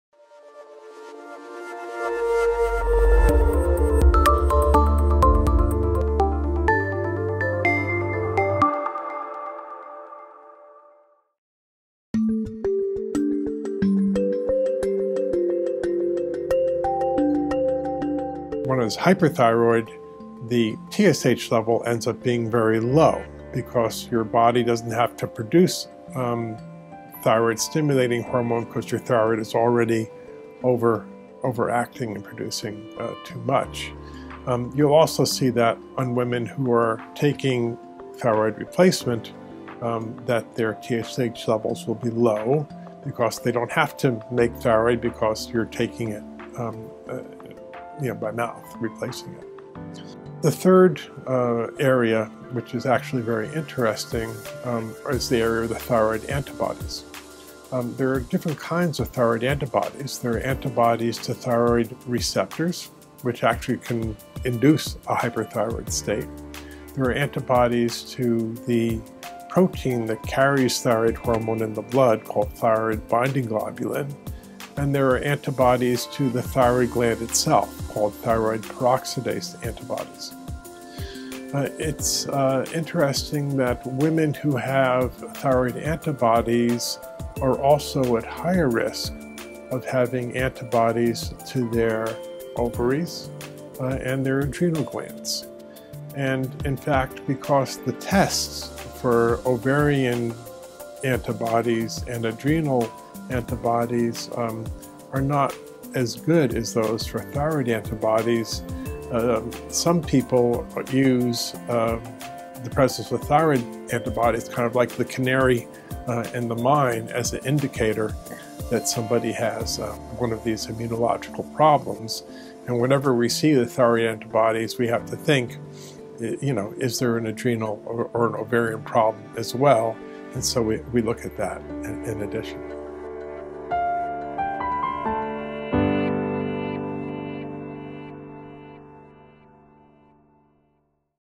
Thyroid Abnormalities Signal Immune Issues & Can Affect Fertility
Presence of thyroid antibodies can be a clue that there may be ovarian problems that require specific fertility treatment, says the Center for Human Reproduction's Dr. David H. Barad.